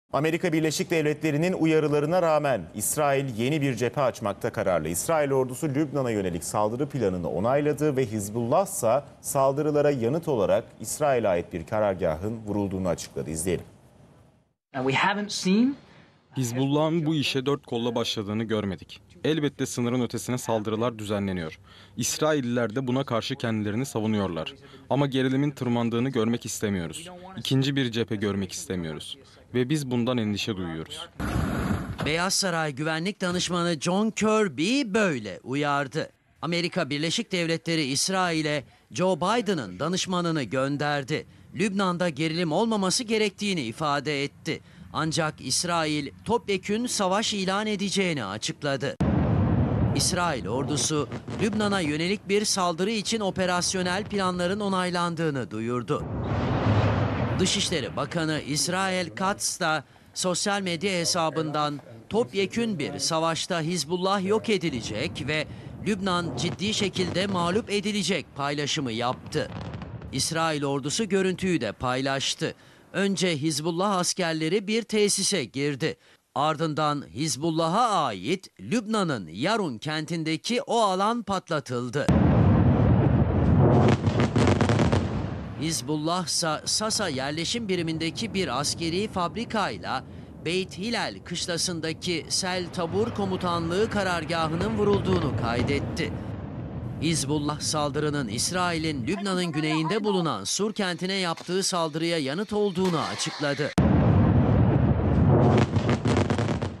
İsrail - Lübnan Hattında Tansiyon Yüksek! "Hizbulla'a Karşı Topyekün Savaş İlanı Yakın"
ABD'nin uyarılarına rağmen İsrail yeni bir cephe açmaya kararlı.

En son haberler, en güncel gelişmeler ve uzman yorumcuların haber analizleri CNN TÜRK ekranında canlı yayınlar ile izleyicisiyle buluşuyor.